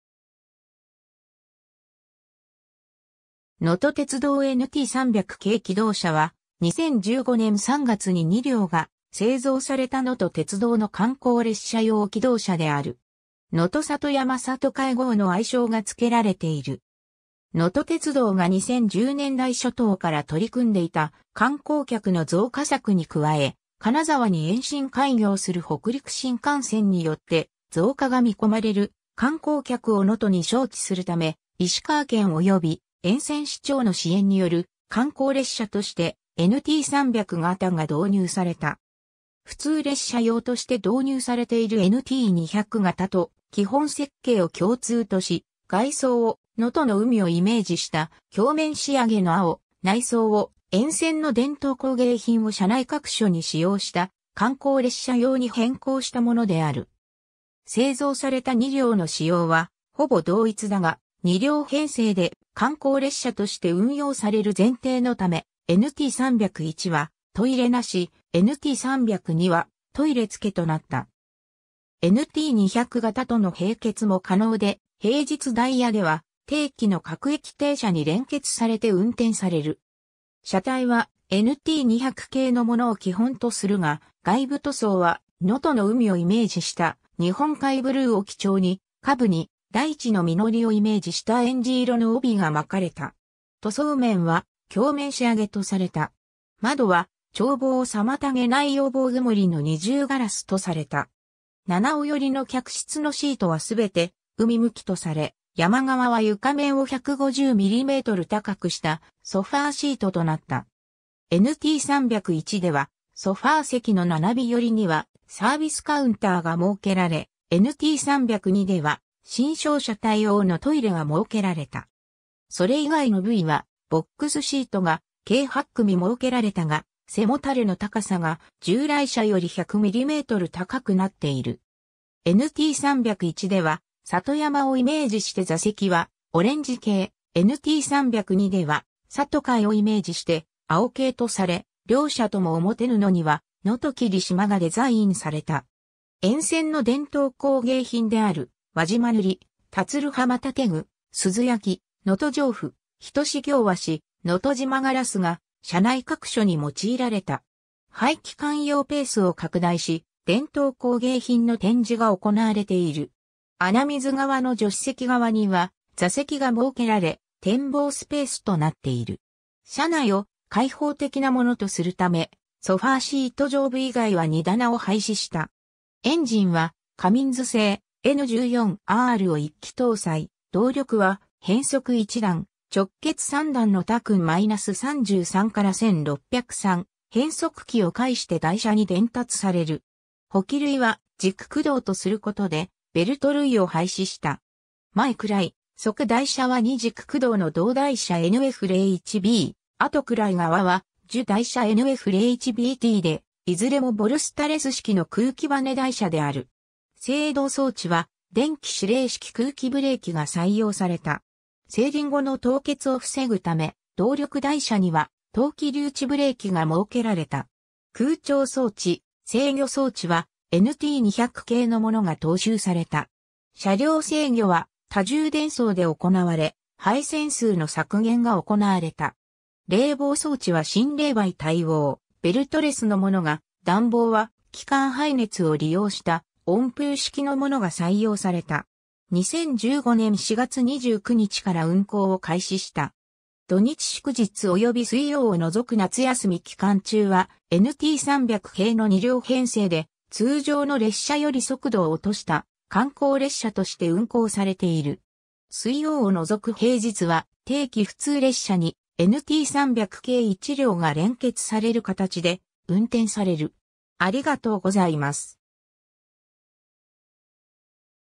のと鉄道NT300形気動車
のと鉄道NT300形気動車（のとてつどうNT300がたきどうしゃ）は、2015年（平成27年）3月に2両が製造されたのと鉄道の観光列車用気動車である。
「のと里山里海号」の愛称がつけられている。
 のと鉄道が2010年代初頭から取り組んでいた観光客の増加策に加え、金沢に延伸開業する北陸新幹線によって増加が見込まれる観光客を能登に招致するため、石川県および沿線市町の支援による観光列車としてNT300形が導入された。
普通列車用として導入されているNT200形と基本設計を共通とし、外装を能登の海をイメージした鏡面仕上げの青、内装を沿線の伝統工芸品を車内各所に使用した観光列車用に変更したものである。
製造された2両の仕様はほぼ同一だが、2両編成で観光列車として運用される前提のため、NT301はトイレなし、NT302はトイレ付となった。
NT200形との併結も可能で、平日ダイヤでは定期の各駅停車に連結されて運転される。
 車体はNT200形のものを基本とするが、外部塗装は能登の海をイメージした「日本海ブルー」を基調に、下部に大地の実りをイメージしたえんじ色の帯がまかれた。
塗装面は鏡面仕上げとされた。
窓は眺望を妨げないよう防曇の2重ガラスとされた。
七尾寄りの客室のシートはすべて海向きとされ、山側は床面を150 mm高くしたソファーシートとなった。
NT301ではソファー席の七尾寄りにはサービスカウンターが設けられ、NT302では身障者対応のトイレが設けられた。
それ以外の部位はボックスシートが計8組設けられたが、背もたれの高さが従来車より100 mm高くなっている。
NT301では「里山」をイメージして座席はオレンジ系、NT302では「里海」をイメージして青系とされ、両車とも表布には「のときりしま」がデザインされた。
沿線の伝統工芸品である輪島塗、田鶴浜建具、珠洲焼、能登上布、仁行和紙、能登島ガラスが車内各所に用いられた。
排気管用ペースを拡大し、伝統工芸品の展示が行われている。
穴水側の助士席側には座席が設けられ、展望スペースとなっている。
車内を開放的なものとするため、ソファーシート上部以外は荷棚を廃止した。
 エンジンは、 カミンズ製N14R（257 kW / 2,000 rpm）を1基搭載、動力は変速1段・直結3段のTACN-33-1603変速機を介して台車に伝達される。
補器類は軸駆動とすることでベルト類を廃止した。
前位側台車は2軸駆動の動台車NF01B、後位側は従台車NF01BTで、いずれもボルスタレス式の空気ばね台車である。
制動装置は電気指令式空気ブレーキが採用された。
制輪子の凍結を防ぐため、動力台車には冬季留置ブレーキが設けられた。
 空調装置、制御装置はNT200形のものが踏襲された。
車両制御は多重伝送で行われ、配線数の削減が行われた。
冷房装置は新冷媒対応、ベルトレスのものが、暖房は機関排熱を利用した温風式のものが採用された。
 2015年（平成27年）4月29日から運行を開始した。
土日祝日および水曜を除く夏休み期間中はNT300形の2両編成で、通常の列車より速度を落とした観光列車として運行されている。
水曜を除く平日は、定期普通列車にNT300形1両が連結される形で運転される。